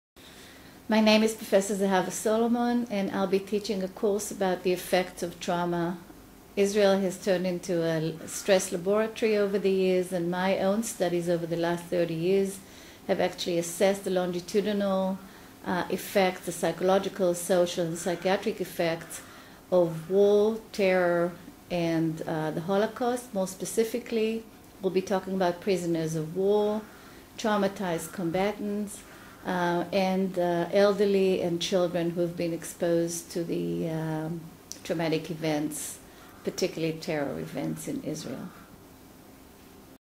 Prof. Zahava Solomon.wmv
Prof. Zahava Solomon
The Bob Shapell School of Social Work at Tel-Aviv University is offering a new M.A. program on Coping with Stress, Crisis and Trauma for international students.
Israel is a living laboratory for research and practice in coping with stress, crisis, and trauma. Over the years, Israeli scholars and practitioners have developed considerable knowledge and models of intervention in these areas. Our academic staff has been at the forefront of these developments. We invite you to learn from our world renowned experts.